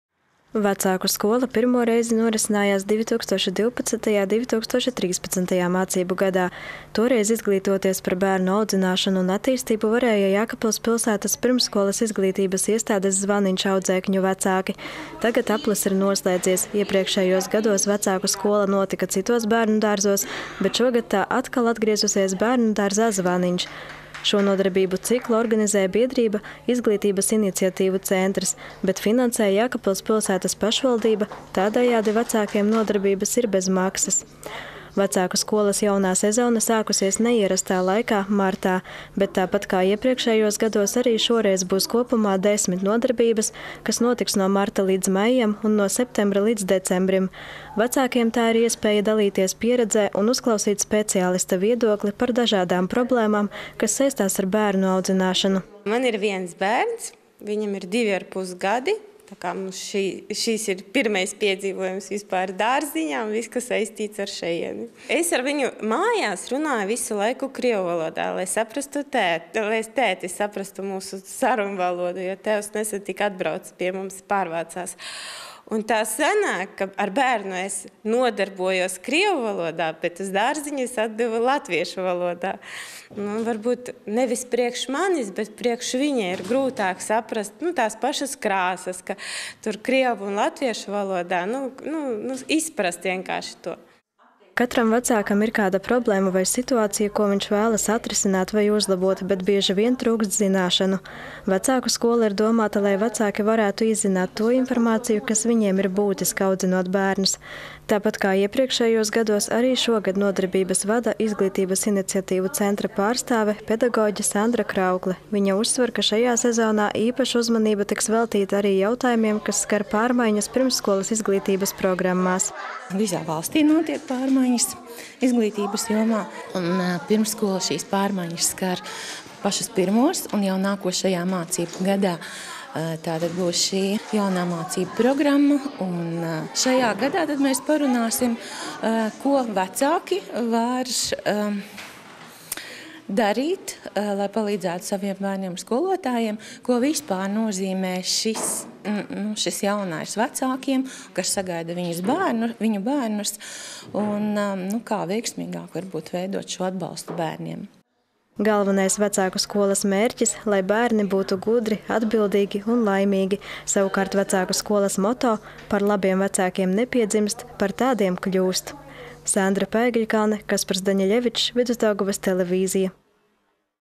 Jēkabpilī atklāj Vecāku skolas jauno sezonu
Vecāku skola pirmo reizi norisinājās 2012./2013.mācību gadā. Toreiz izglītoties par bērnu audzināšanu un attīstību varēja Jēkabpils pilsētas pirmsskolas izglītības iestādes “Zvaniņš” audzēkņu vecāki. Tagad aplis ir noslēdzies. Iepriekšējos gados Vecāku skola notika citos bērnudārzos, bet šogad tā atkal atgriezusies bērnudārzā “Zvaniņš”. Šo nodarbību ciklu organizē biedrība “Izglītības iniciatīvu centrs”, bet finansē – Jēkabpils pilsētas pašvaldība, tādējādi vecākiem nodarbības ir bez maksas. Vecāku skolas jaunā sezona sākusies neierastā laikā – martā, bet, tāpat kā iepriekšējos gados, arī šoreiz būs kopumā 10 nodarbības, kas notiks no marta līdz maijam un no septembra līdz decembrim. Vecākiem tā ir iespēja dalīties pieredzē un uzklausīt speciālista viedokli par dažādām problēmām, kas saistās ar bērnu audzināšanu.

    Jēkabpiliete Žanna, kura ieradusies uz pirmo jaunās sezonas nodarbību, stāsta: “Man ir viens bērns, viņam ir divarpus gadi, šī ir mūsu pirmā pieredze bērnudārzā. Ar bērnu mājās es runāju krievu valodā, lai tētis saprastu mūsu sarunas, jo tētis tikai nesen pie mums pārvācās. Sanāk, ka ar bērnu es nodarbojos krievu valodā, bet dārziņā ar viņu strādā latviešu valodā. Viņam ir grūtāk saprast, atšķirt vārdus krievu valodā un latviešu valodā.”

    Katram vecākam ir kāda problēma vai situācija, ko viņš vēlas atrisināt vai uzlabot, bet bieži vien trūkst zināšanu. Vecāku skola ir domāta, lai vecāki varētu izzināt to informāciju, kas viņiem ir būtiska, audzinot bērnus. Tāpat kā iepriekšējos gados, arī šogad nodarbības vada Izglītības iniciatīvu centra pārstāve, pedagoģe Sandra Kraukle. Viņa uzsver, ka šajā sezonā īpaša uzmanība tiks veltīta arī jautājumiem, kas skar pārmaiņas pirmsskolas izglītības programmās: “Visā valstī notiek pārmaiņas izglītības jomā, un pirmsskolu šīs pārmaiņas skar pirmo. Jau nākošajā mācību gadā būs jaunā mācību programma, tādēļ šajā gadā mēs parunāsim par to, ko vecāki var darīt, lai palīdzētu saviem bērniem un skolotājiem, ko nozīmē jaunās pārmaiņas, kas sagaida bērnus un kā veiksmīgāk veidot atbalstu bērniem.”

    Galvenais Vecāku skolas mērķis – lai bērni būtu gudri, atbildīgi un laimīgi, savukārt Vecāku skolas moto: “Par labiem vecākiem nepiedzimst, par tādiem kļūst!”.

    Sandra Paegļkalne